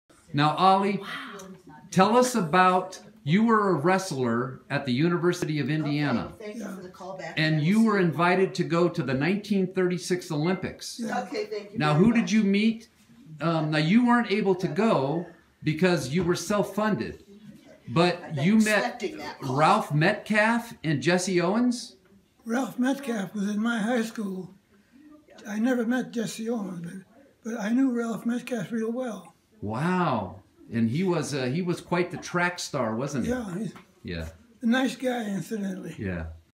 Ollie and Ralph Metcalfe
(Recorded Feb 10, 2017 in Colorado Springs, 104th birthday) Ollie went to high school with Ralph Metcalfe. Metcalfe was a silver medalist at the 1932 Olympics games in Los Angeles and a gold medalist in the 1936 Berlin Olympics. Ollie was a wrestler at the University of Indiana and was invited to the '36 games but couldn't afford to attend.